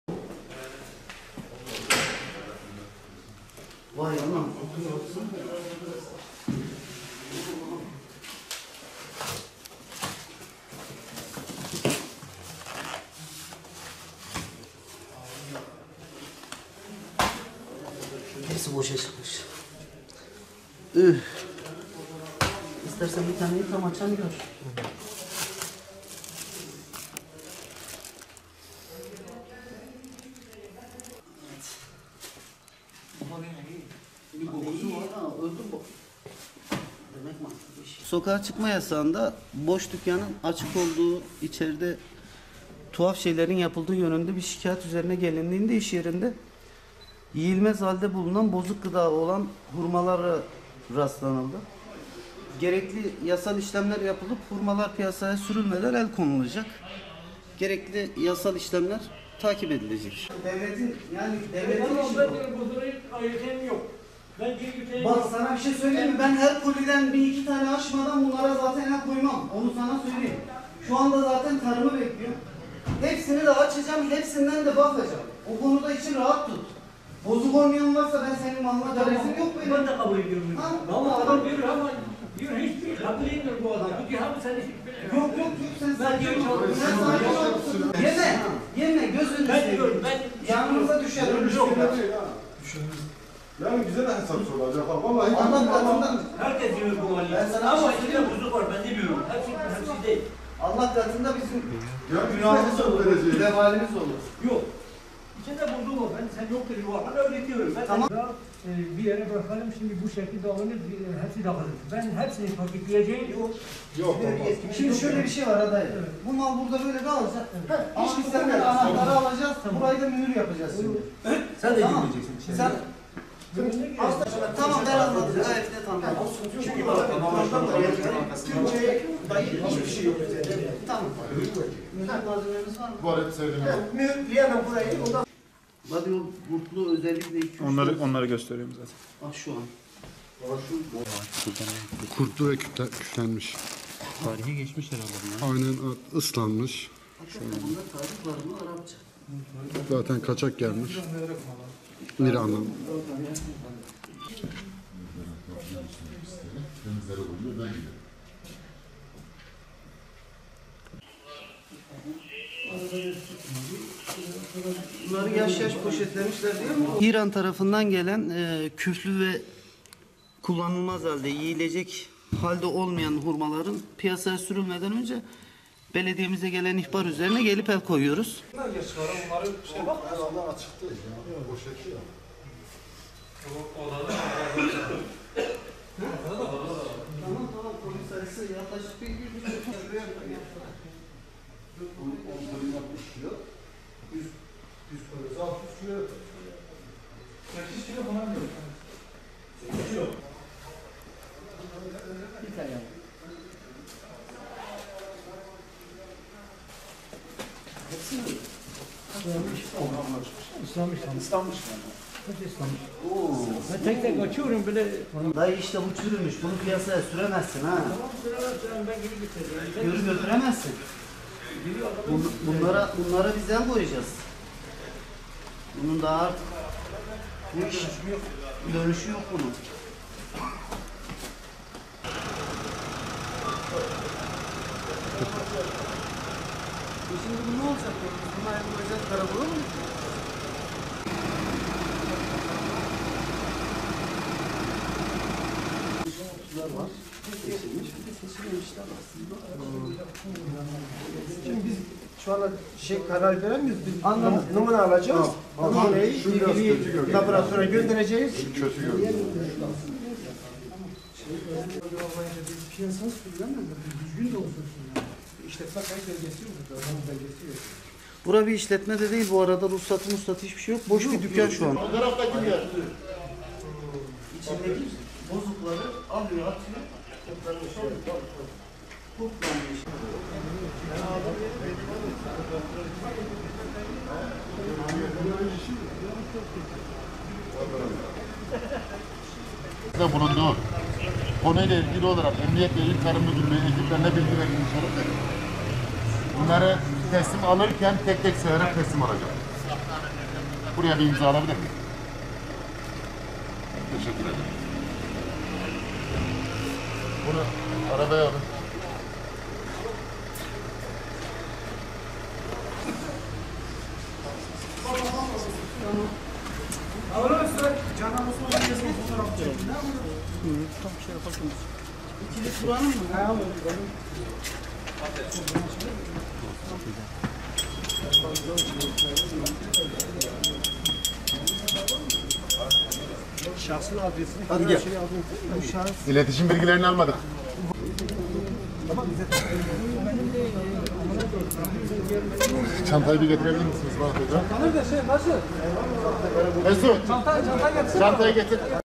1,5 ton bozulmuş hurma ele geçirildi, sahibi bakın neler yaptı?
Elazığ'da gelen ihbarı değerlendiren zabıta ekipleri boş bir dükkanda, bozuk ve küflenmiş 1,5 ton hurma ele geçirdi.